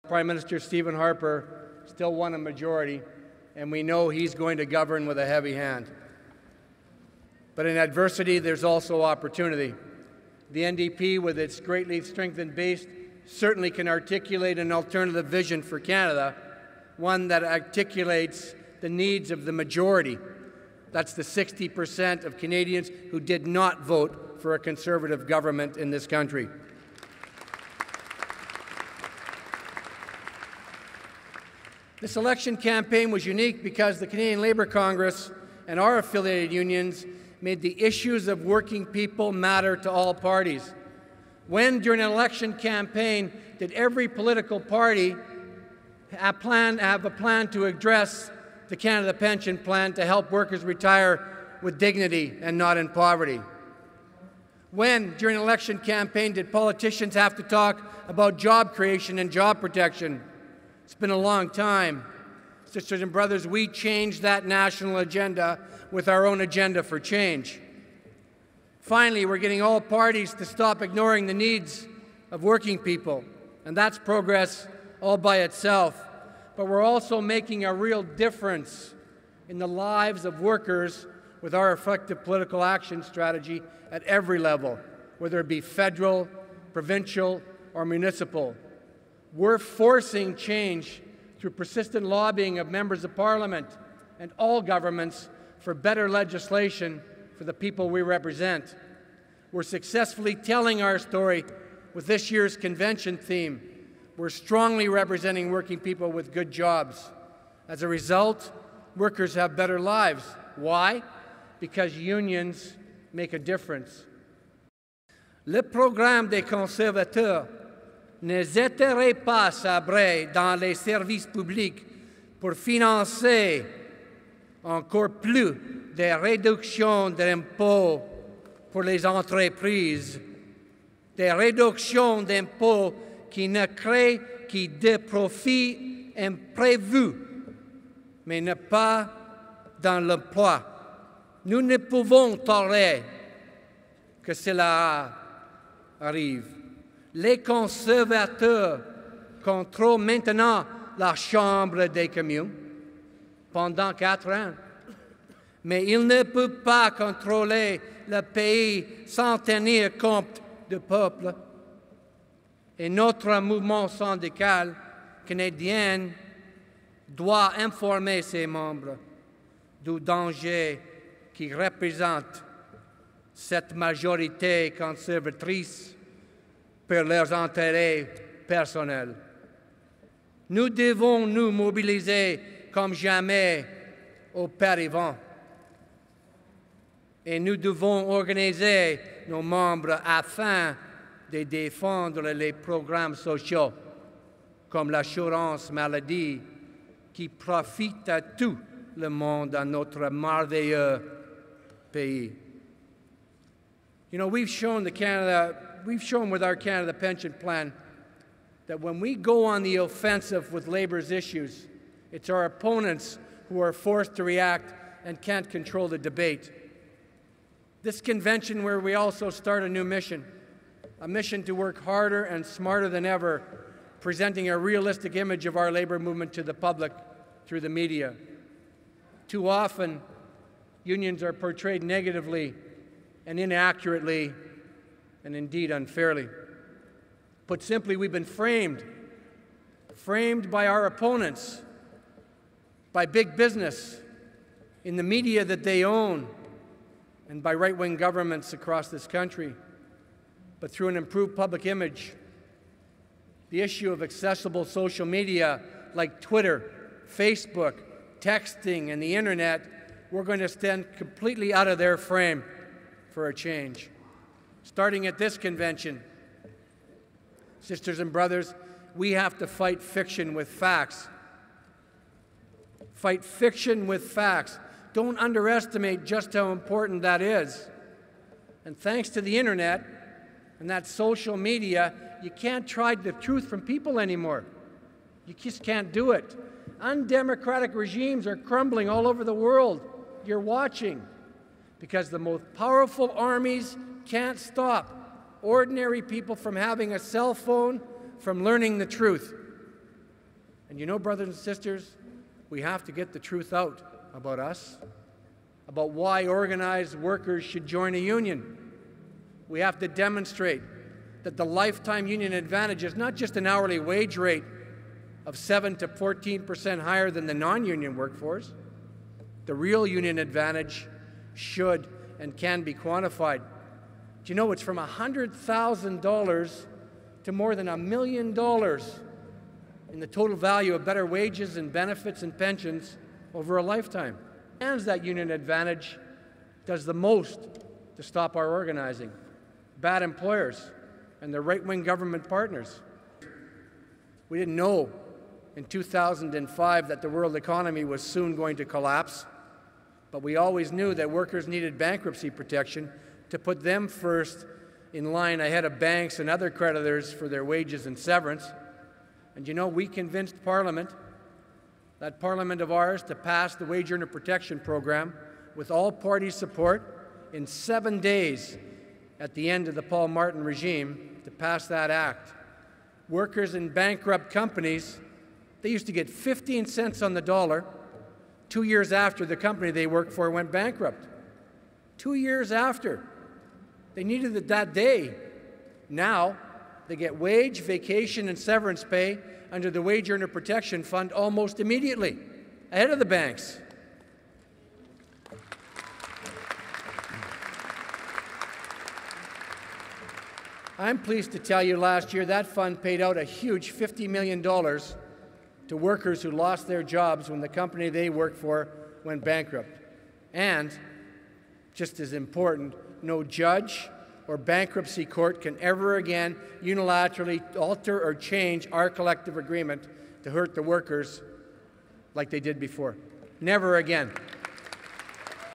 Welcome to Convention - Good Jobs, Better Lives - Ken Georgetti - Part 1
President Ken Georgetti welcomes delegates to the CLC Convention in Vancouver and talks abou the CLC's success and the future action plan.